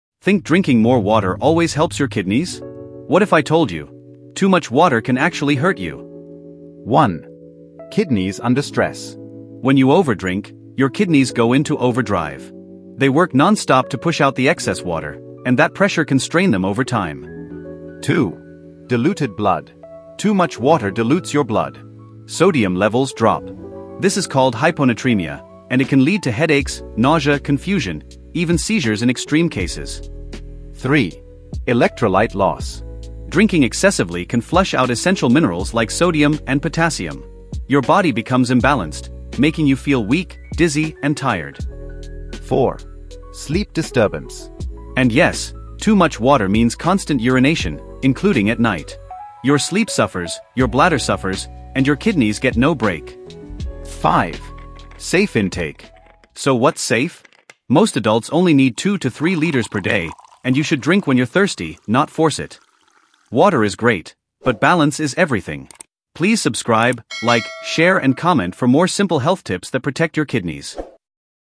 The Dangers of Drinking Too Much Water
Drinking water is healthy, but too much water can actually stress your kidneys, dilute your blood, and create dangerous electrolyte imbalances.
In this short video, we break down the effects of overhydration and how to drink water safely every day.

If you care about kidney health, hydration, and lifestyle wellness, this video is for you.